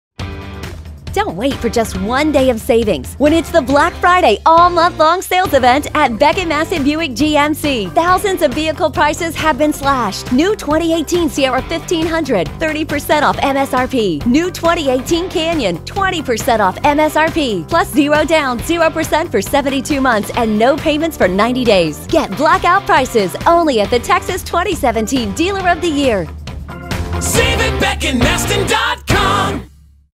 Black Friday All Month Long - Spot 5
DON’T WAIT FOR JUST ONE DAY OF SAVINGS
AT BECK & MASTEN BUICK GMC
DURING THE BLACK FRIDAY ALL MONTH LONG SALES EVENT
THOUSANDS OF VEHICLE PRICES HAVE BEEN SLASHED
PLUS $0 DOWN 0% FOR 72 MONTHS AND NO PAYMENTS FOR 90 DAYS
SAVE MORE THAN EVER WITH OUR BLACKOUT PRICES
ONLY AT THE TEXAS 2017 DEALER OF THE YEAR